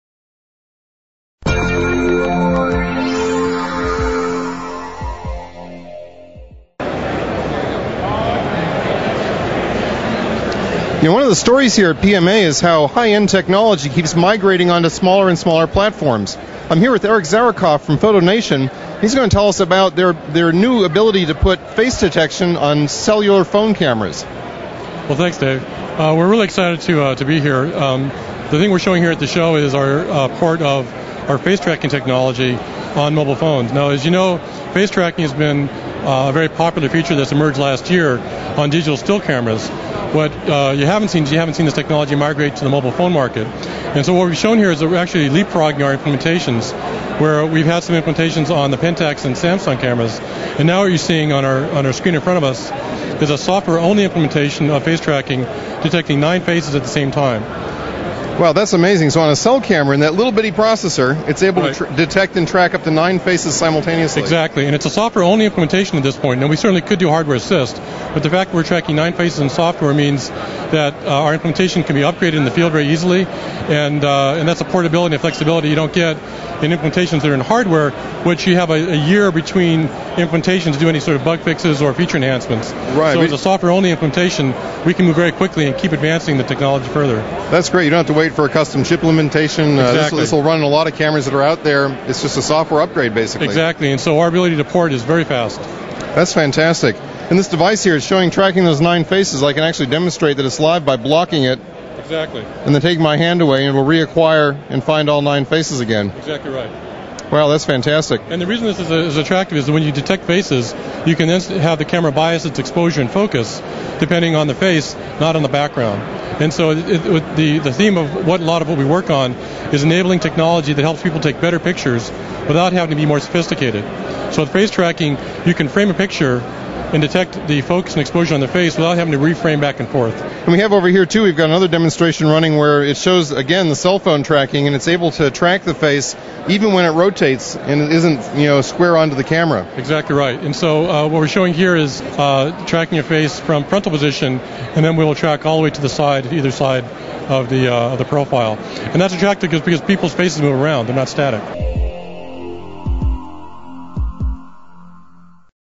The Imaging Resource at PMA 2007: FotoNation Face Detection
Dave Etchells talks to Eric Zarakov from FotoNation about cell phone camera face detection technology. Go to imaging-resource.com for more PMA 2007 coverage.